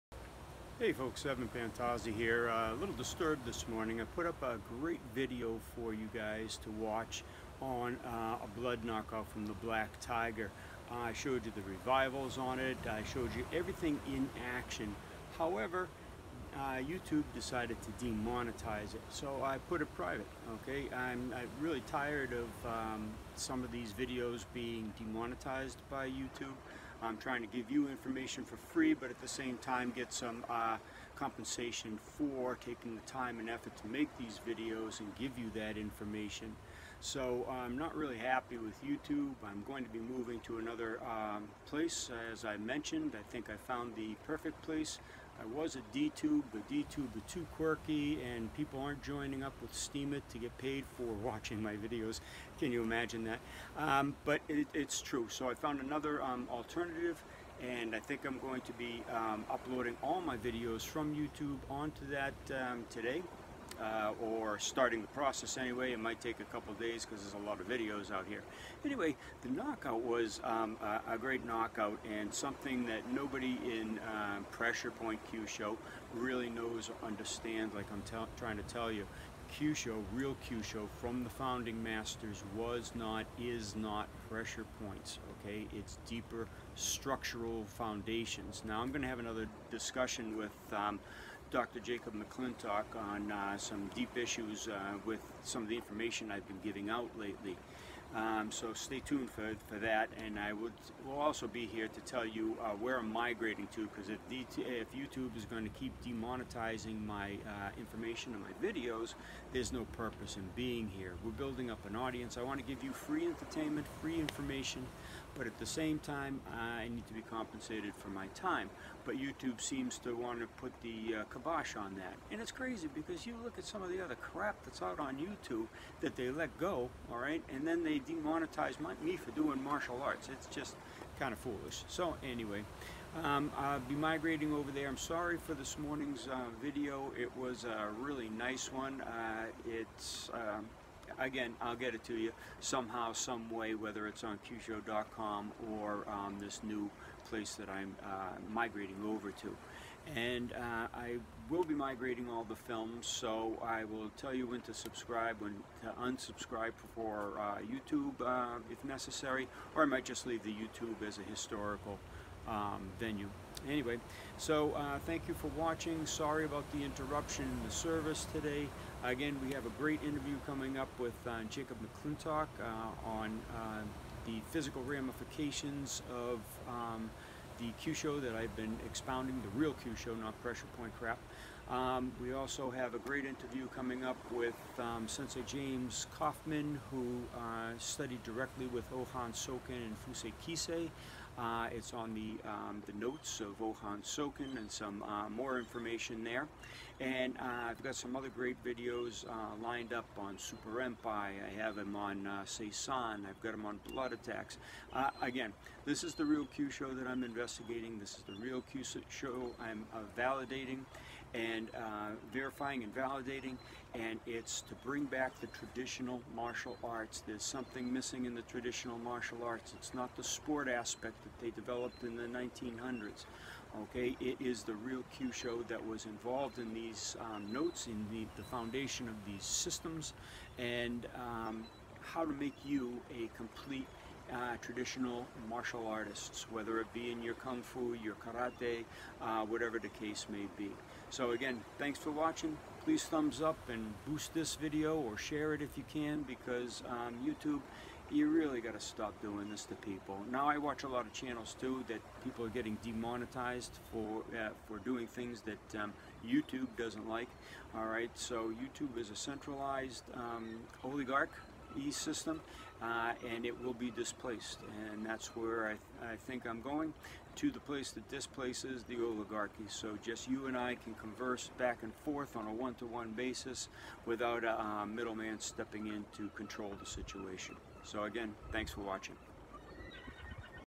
Video Removed
This morning I placed a great video on with deep Kyusho application and KO, but YouTube demonetized it, So I am moving it to a safer place that supports content creators... more soon.

for over 80 full length videos with history, breakdown, analysis, demonstration and training approaches.